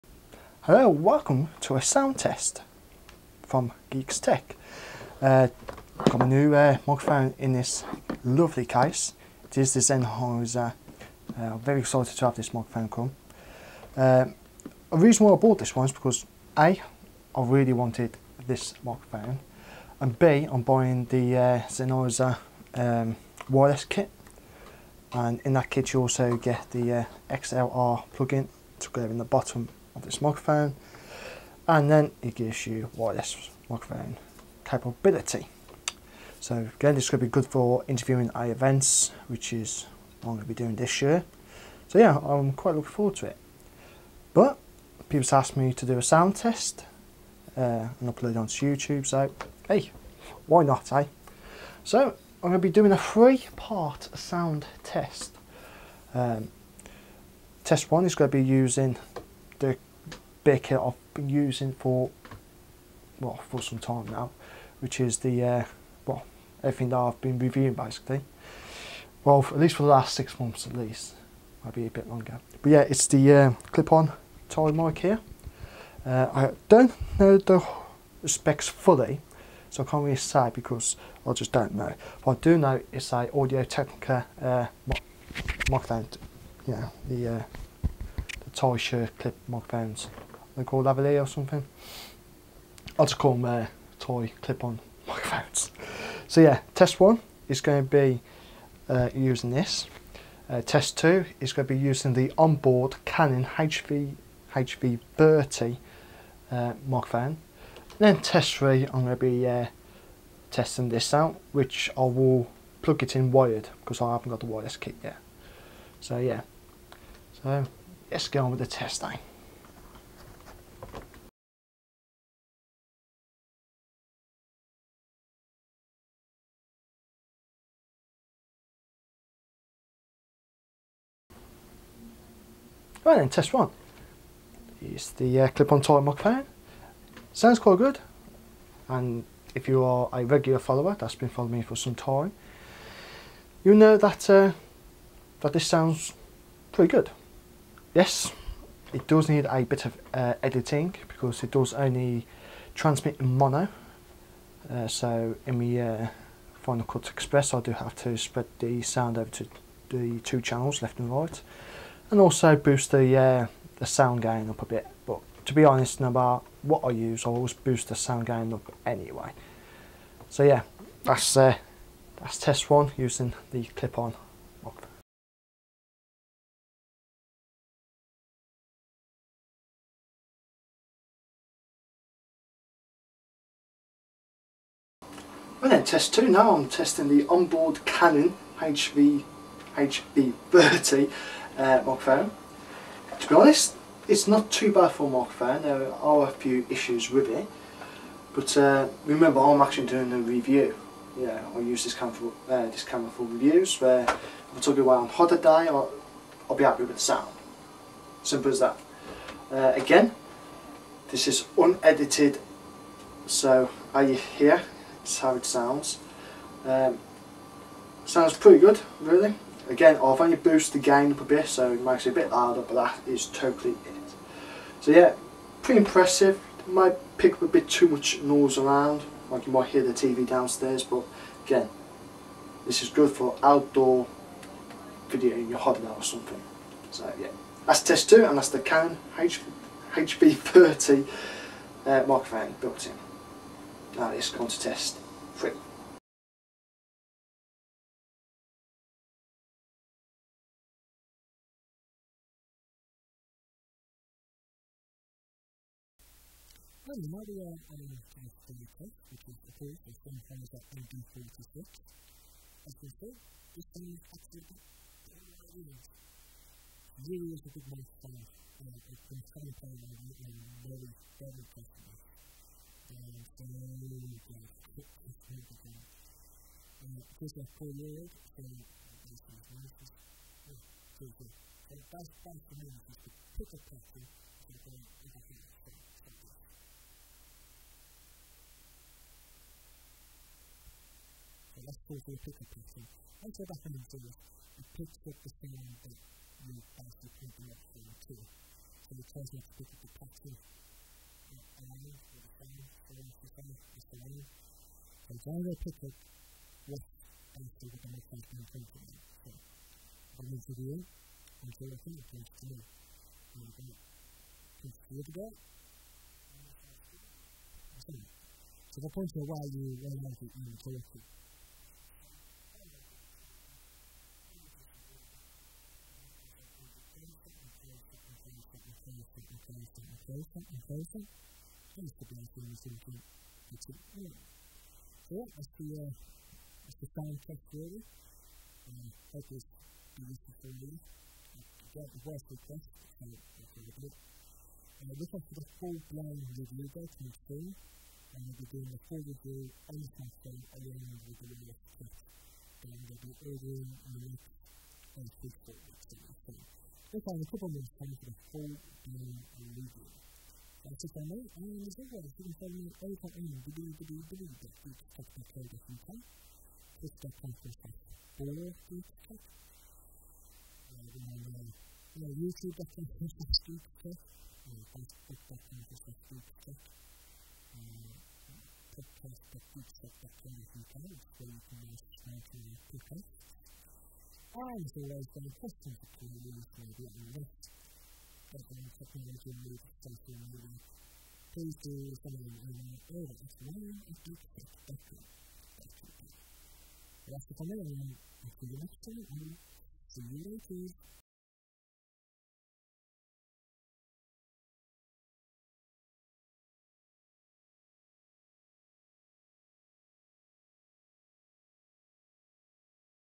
Sound Test: Sennheiser MD46
This is a sound test video, showing you the sound quietly of the Sennheiser MD46